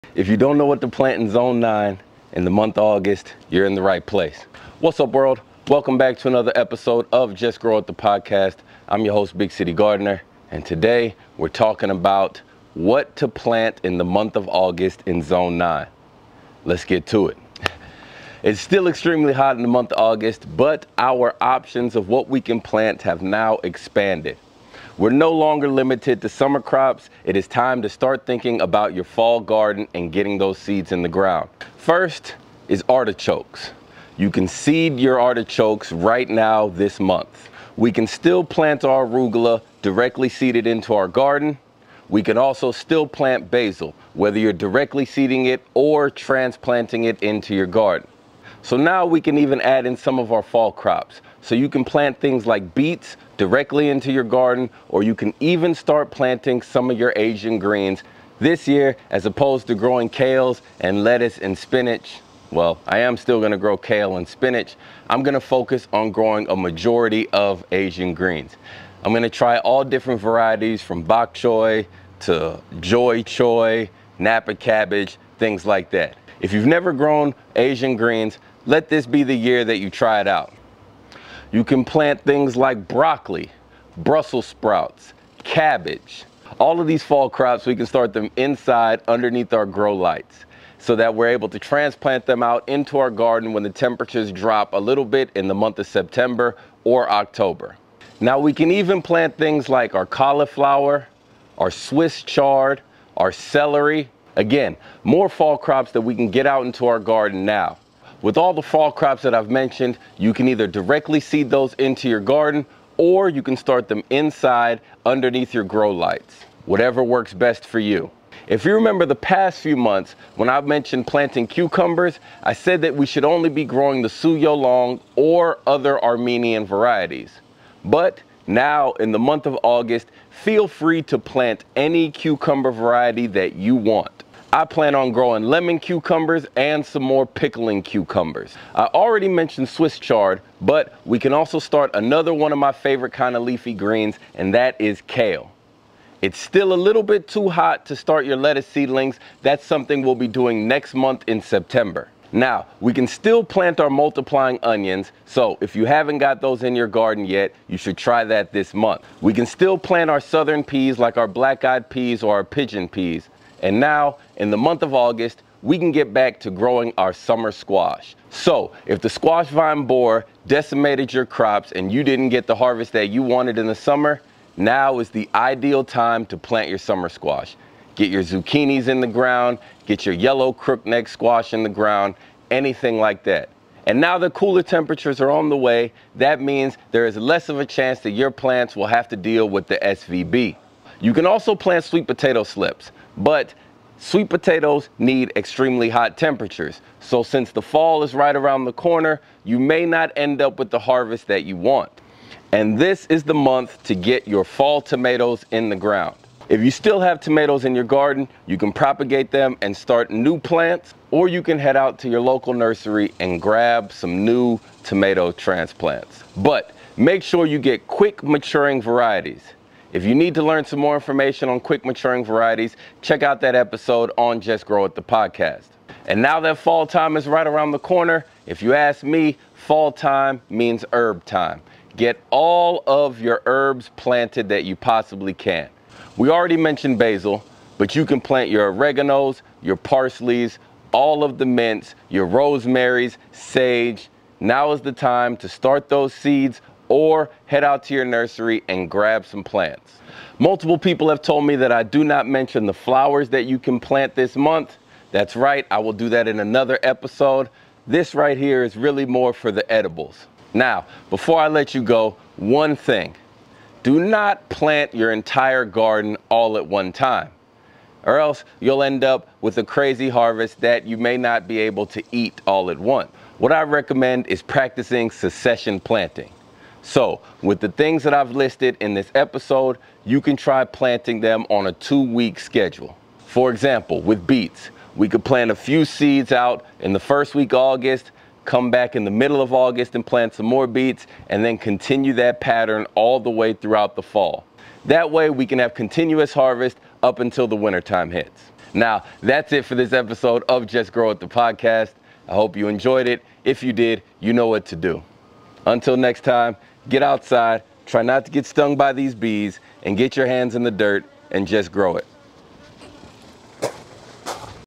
What can I plant this month? August Planting Schedule : Zone 9
Are you wondering what you can grow this month? What should I be planting in August? This video has you covered

Zone 9 is a tricky zone to garden in. Due to the forgiving climate, you can garden 12 months out of the year.   But make sure you plant on time because certain crops have short planting windows.

Check out the planting schedule videos if you want to garden year-round, harvesting something fresh daily.